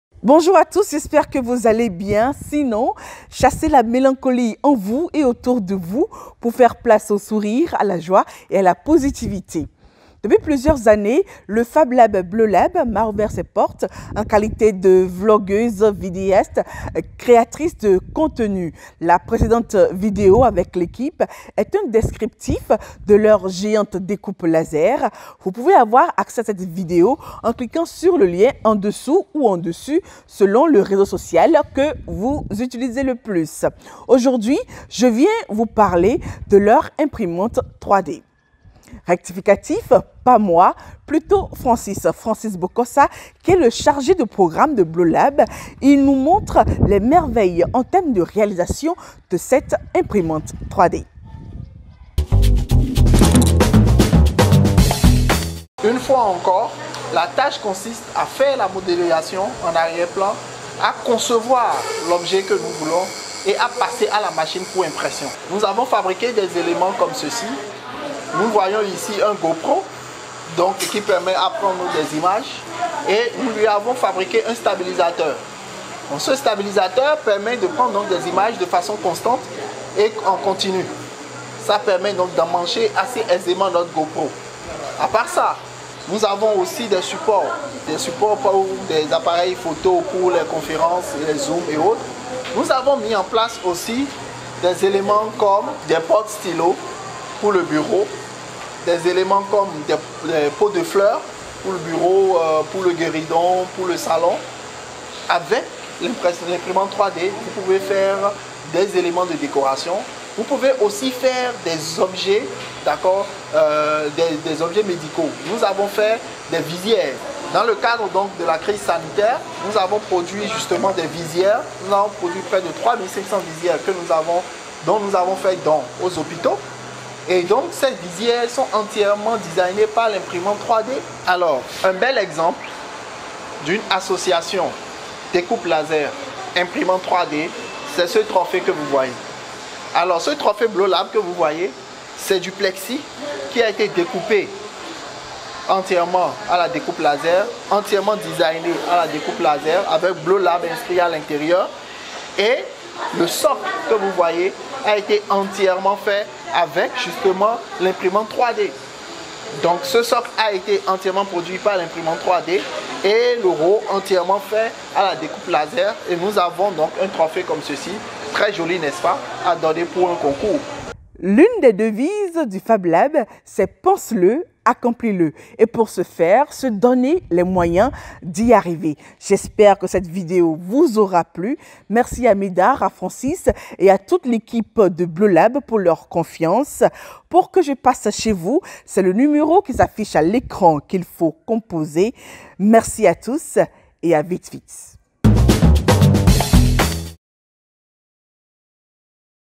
BloLab : un FabLab qui s'inscrit dans la culture du maker ( 2-l'imprimante 3D)
Une visite dans les locaux du FabLab BloLab non loin de la pharmacie Kindonou dans les environs du Stade de l'Amitié Mathieu Kérékou et vous comprendrez mon enthousiasme à faire cette production. Pour m'imprégner des lieux et comprendre ce qui s'y fait, je décide d'abord de m'intéresser à la géante découpe laser de la maison. Je n'ai pas du tout manqué la bonne occasion de me faire faire deux portes clés en mon nom. Ne dit-on pas que ''qui travaille à l'hôtel, vit de l'hôtel"! La vidéo qui en parle est juste ensuite à l'imprimante 3D.  J'avoue qu'avant mon départ, j'ai menacé mais gentiment de partir avec plusieurs des outils réalisés grâce à la découpe laser et à l'imprimante 3D. Ce n'est pas pour rien, j'étais juste impressionnée par toute la passion et le travail quotidien que produit l'équipe du FabLab BloLab chaperonnée par Médard Agbayazon, le maitre des lieux. PLusieurs articles sur le blog dont je laisse l'adresse ) sont d'ailleurs écrit sur lui et sur les activités.
Je ne vous dirai pas plus, je vous laisse juste admirer et applaudir les efforts du FabLab BloLab à travers l'imprimante 3D.